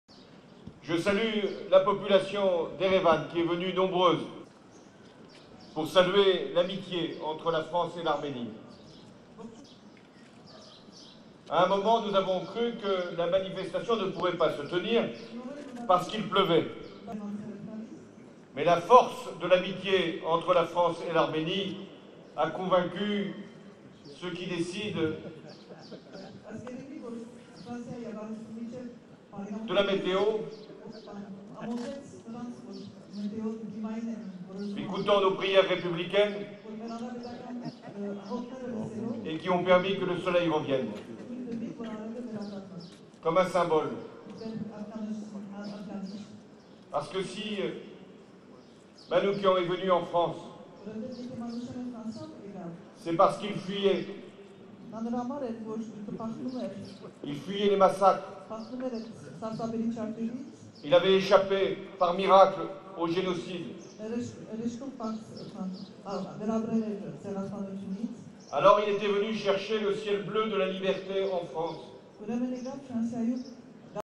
Les "prières républicaines" de Hollande contre la pluie - 13/05
François Hollande s'est laissé à un nouveau commentaire sur la pluie mardi à Erevan, la capitale arménienne, alors qu'il inaugurait un square Missak-Manouchian, du nom de ce résistant communiste exécuté en 1944.